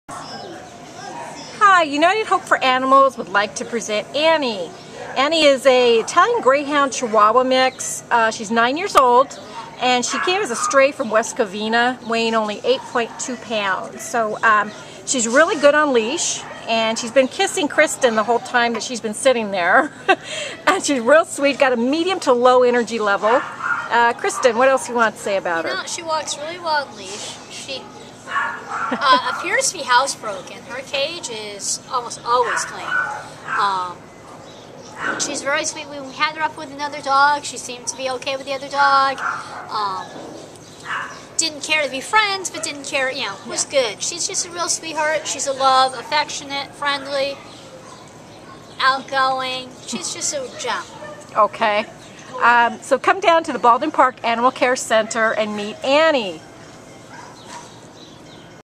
A5039322 Annie | Chihuahua/Italian Greyhound
A5039322 Annie is a stunning, long-legged 9-year-old chocolate-and-white spayed female Chihuahua and Italian Greyhound mix who ended up at the Baldwin Park Animal Care Center on March 8th as a stray from West Covina. Weighing 8 lbs, this small lass is nothing short of statuesque, and she has long dainty legs, a delicateness, and captivating brown eyes. She’s also good on leash, shows signs of being housebroken, is okay with other dogs (she seems to prefer to ignore them, at least in the scary shelter situation), and enjoys human company—she offers sweet and soft affection along with respectable, ladylike kisses. Annie will thrive and enjoy her many years of life ahead in a forever family whose members respect her small size, keep her safe as an indoor pet and companion, and appreciate the joys of a middle-aged lady who’s experienced in love.